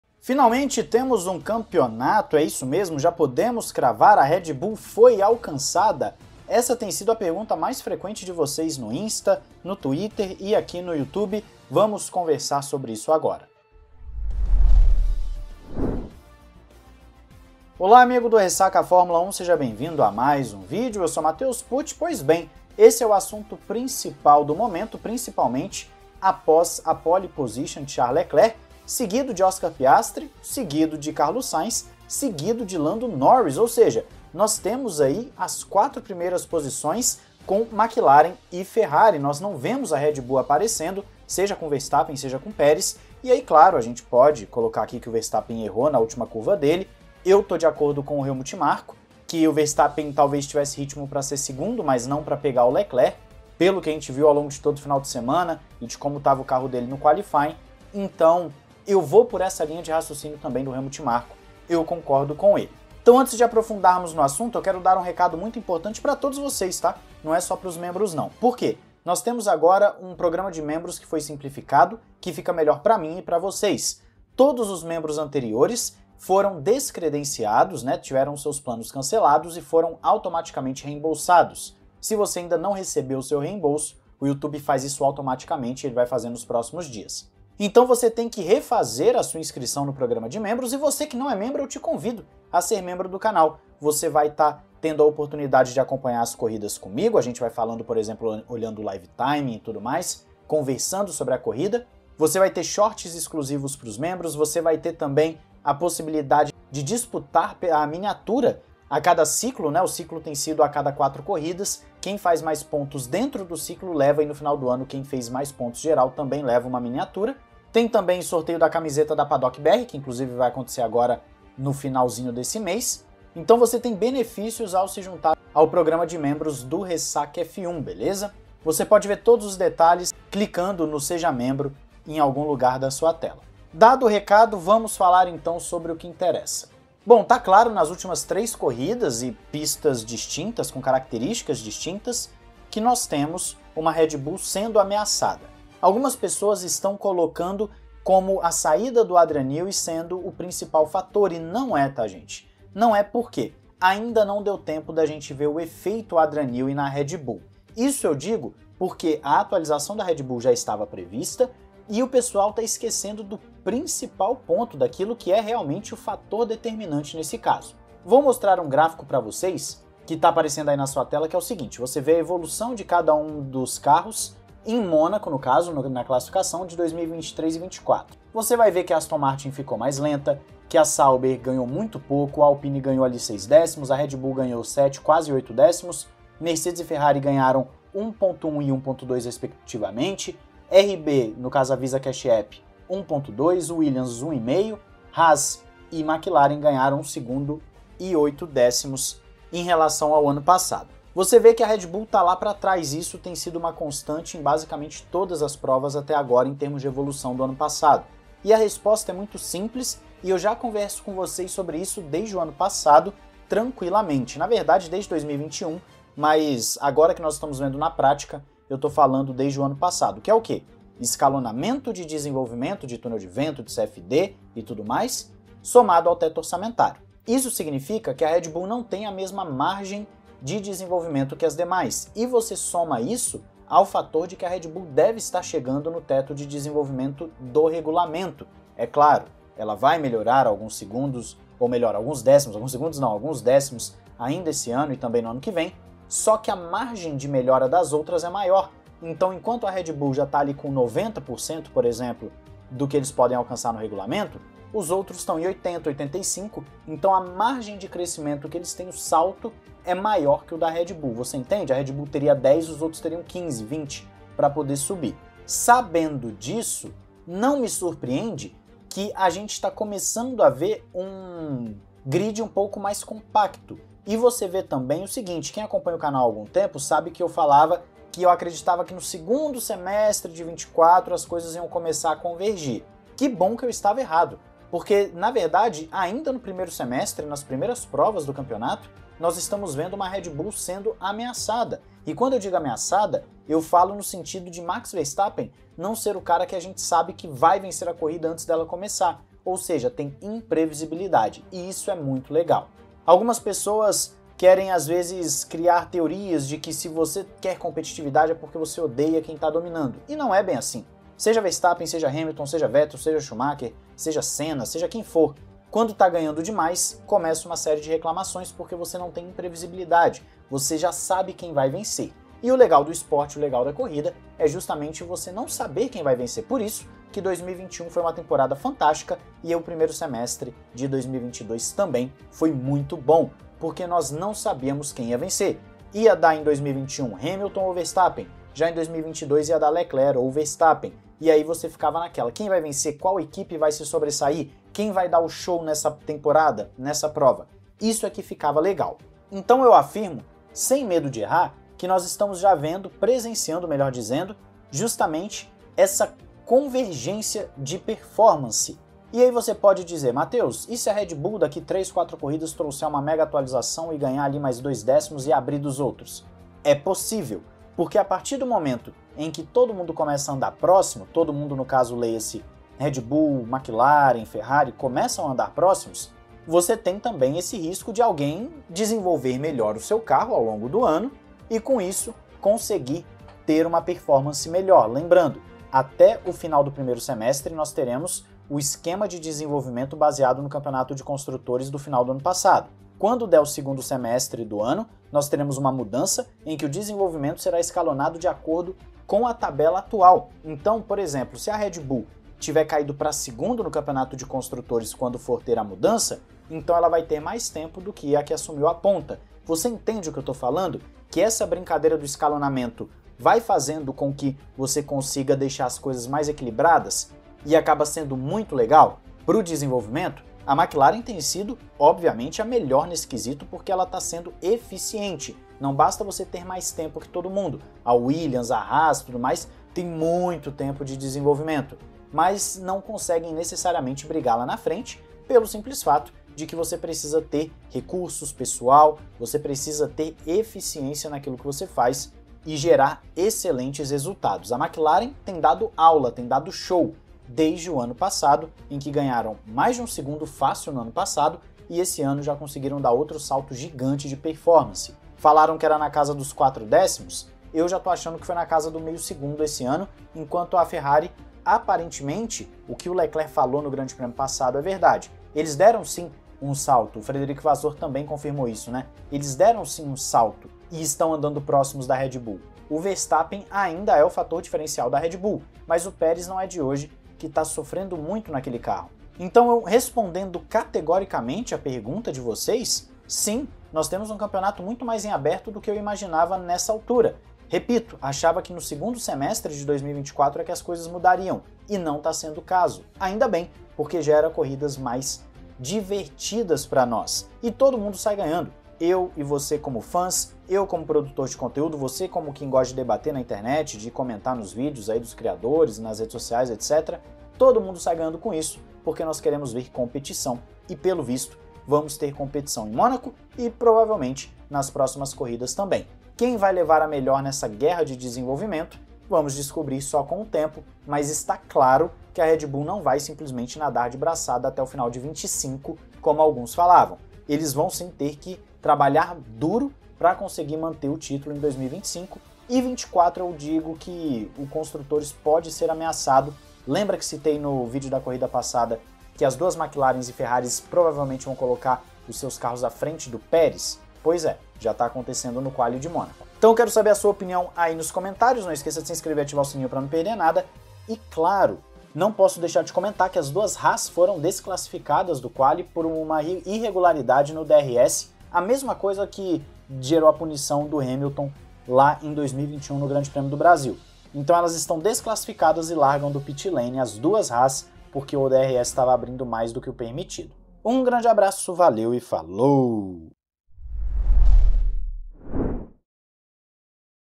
ACABOU O DOMÍNIO MESMO? RED BULL SOFRE PELA 3ª PROVA CONSECUTIVA E CAMPEONATO GANHA FOLEGO
Sergio Milani
@scuderiamilani (Twitter)

📰 *Fontes:*

João 3:16

00:00 - Introdução
00:22 - Temos campeonato
01:04 - programa de membros melhorado
02:15 - Red Bull ameaçada, mas não por conta do Newey
05:51 - COnvergencia de performance
08:00 - Surpreendentemente temos um bom 2024
09:11 - Haas desclassificada do quali